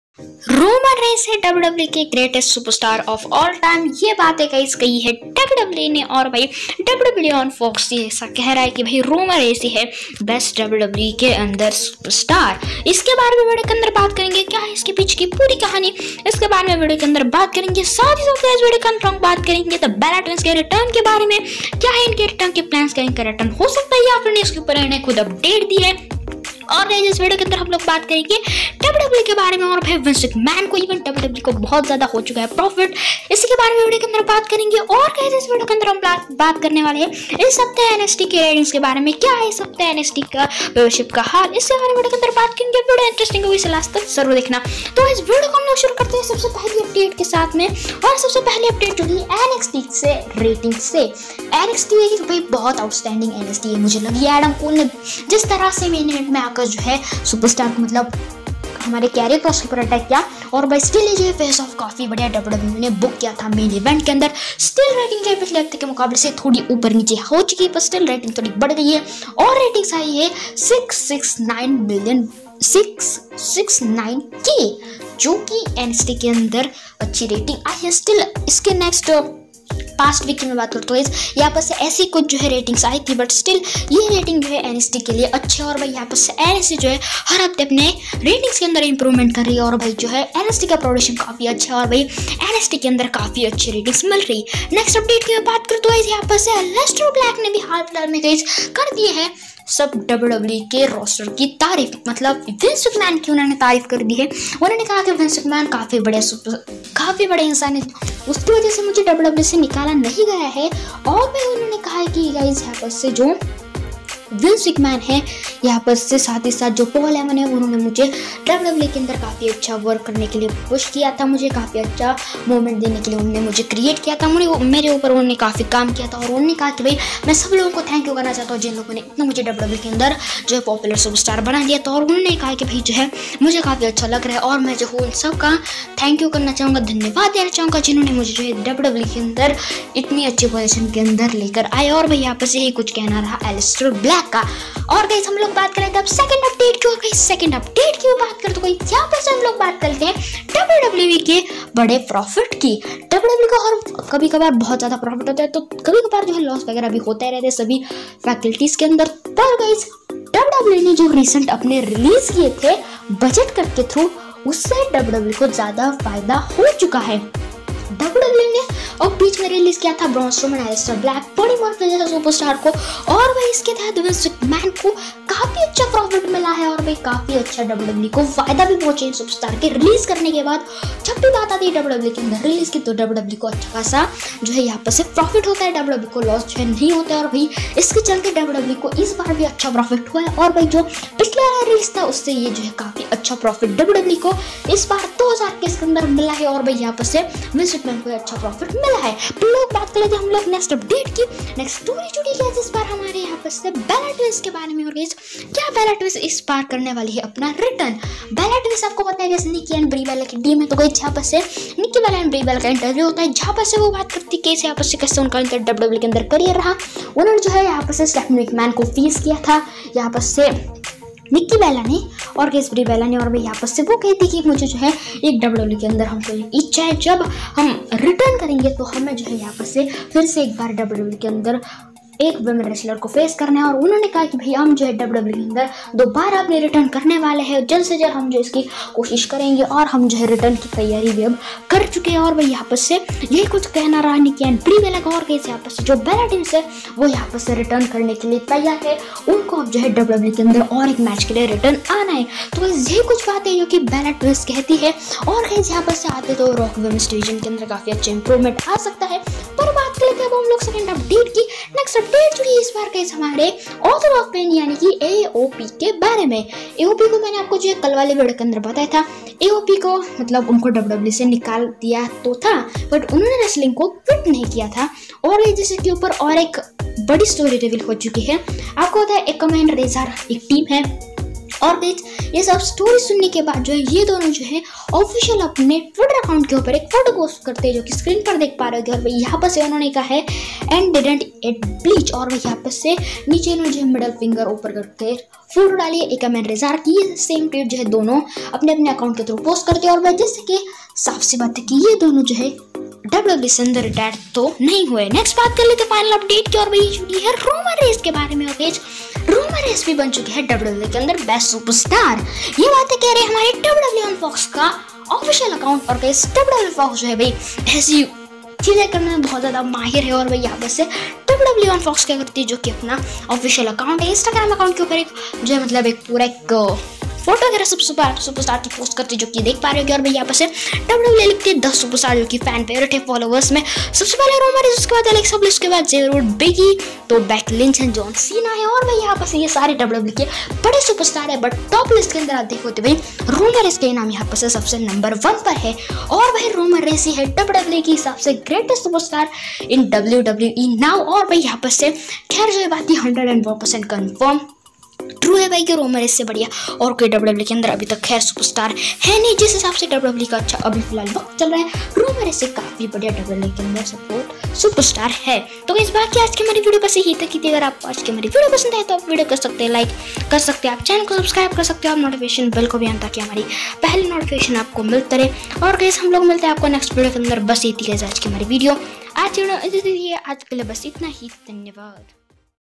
Roman Reigns is Greatst Superstar in WWE ! WWE Got Huge Profits..... Alister Black Thank you all
1)  Roman Reigns is greatest WWE superstar
2)  WWE got huge profit
3) Alister black thank you all WWE Universe
4) the Bell twins rerunning update
5) Nxt This week ratings